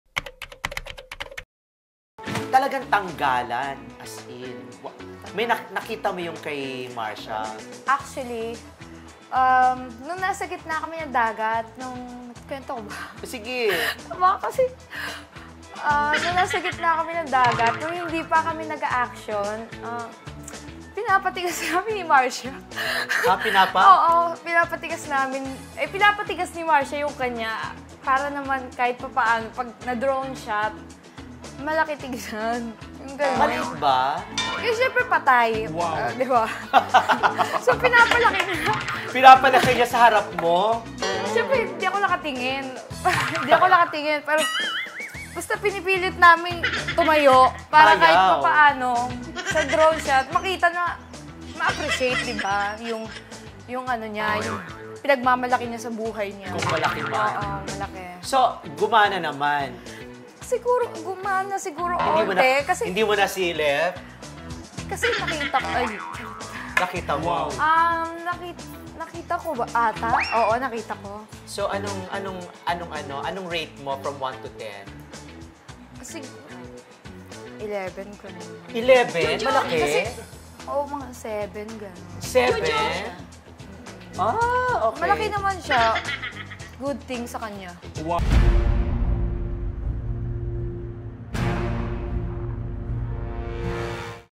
Vivamax actor binubuhay ‘batuta’ bago eksena | ABANTELLILING QUICKIE
“MALAKI NAMAN SIYA”

Binuking ni Vivamax actress Julia Victoria ang kanyang partner sa ‘Sisid Marino’ na si John Mark Marcia na sa kanilang sexy scene sa bangka ay pinapatigas nito ang kanyang ‘alaga’ bago ang action.